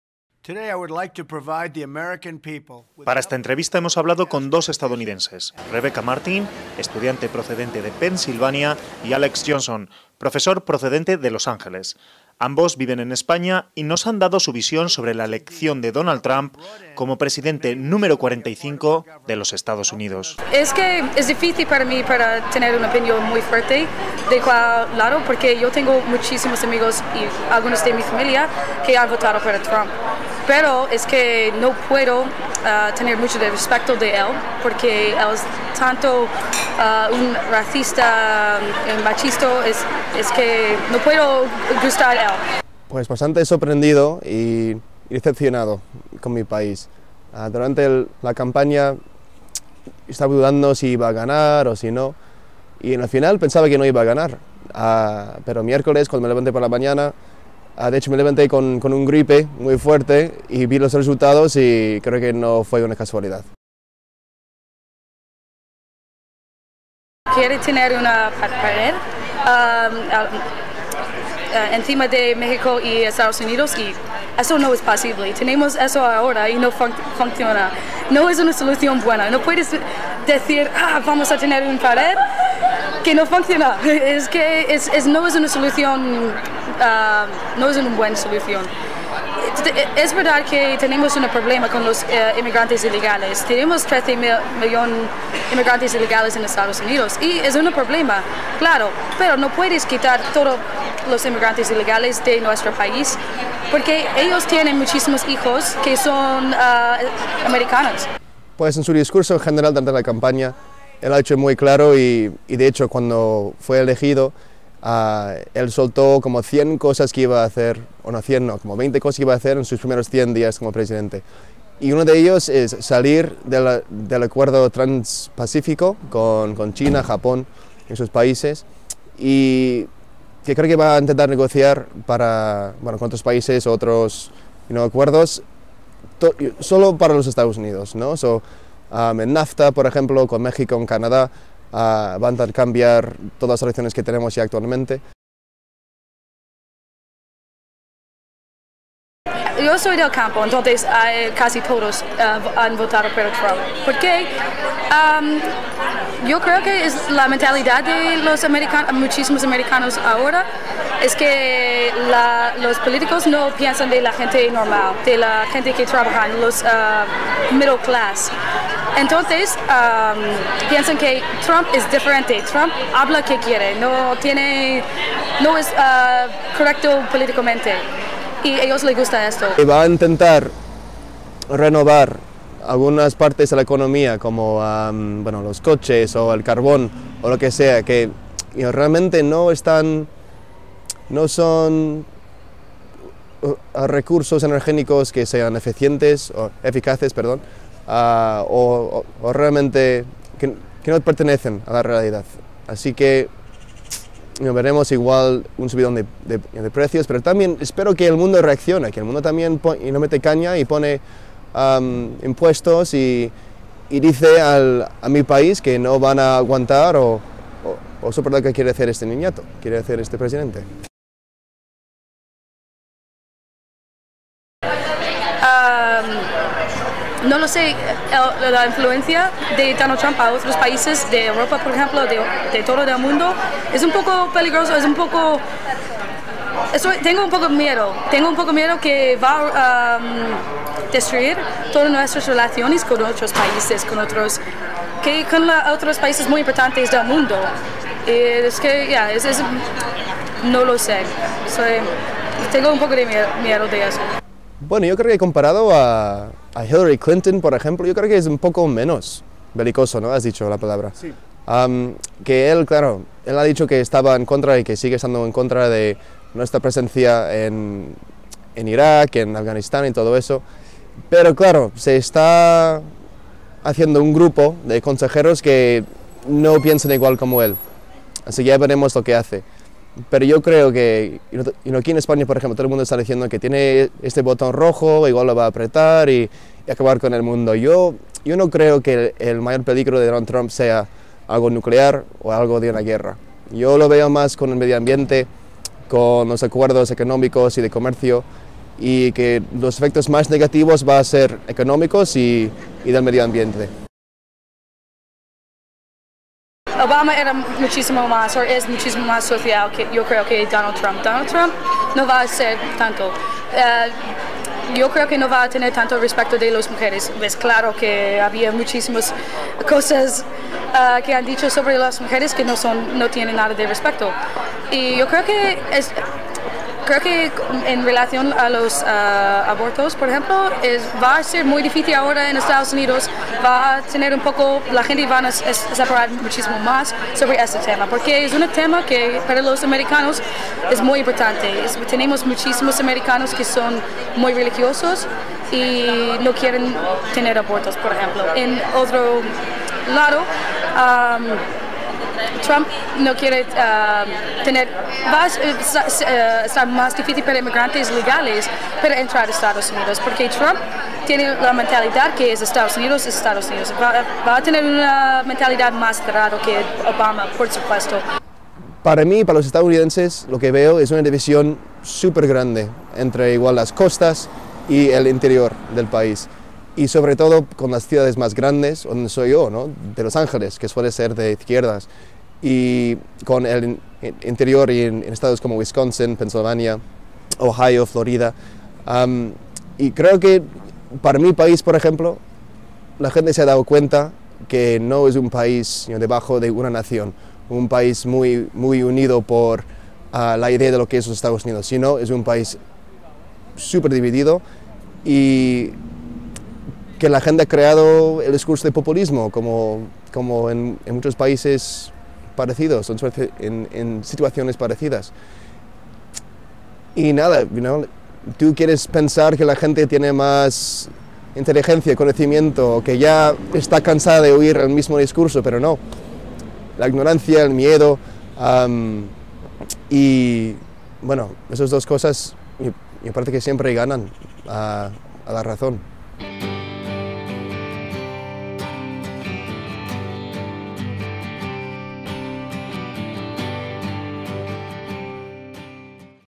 ¿¿¿Bienvenido Mr...... Trump???
Hablamos con dos ciudadanos estadounidenses residentes en Zaragoza sobre la elección de Donald Trump como Presidente de su país.